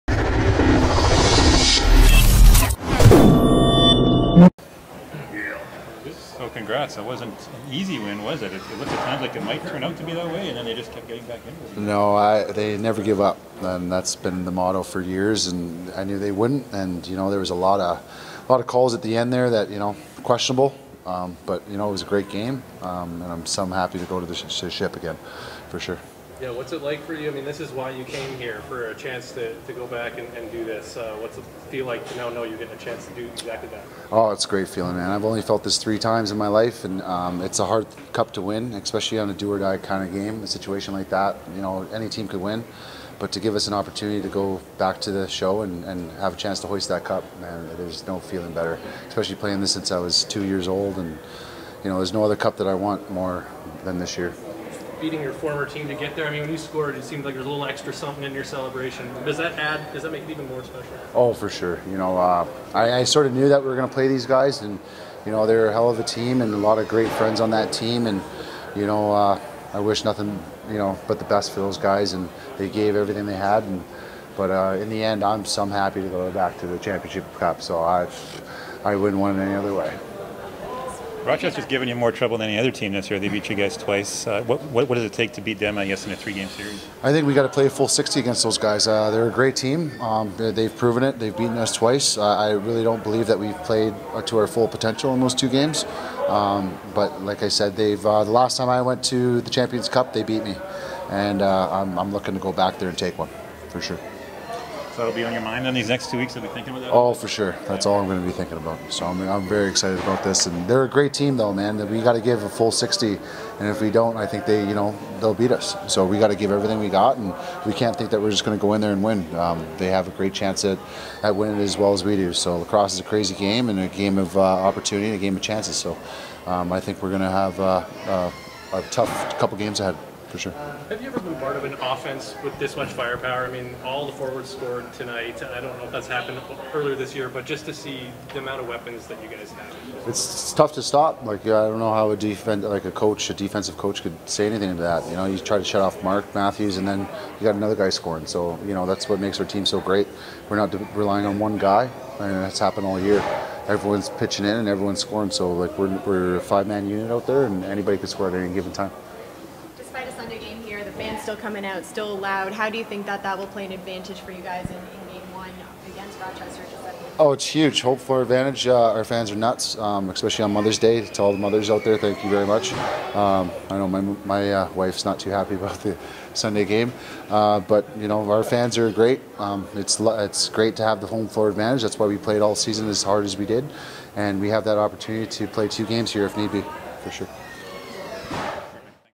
May 13 | Post-game: Jeff Shattler
Hear from Rush's Jeff Shattler after beating his former team, the Roughnecks, to move on to the NLL Championship.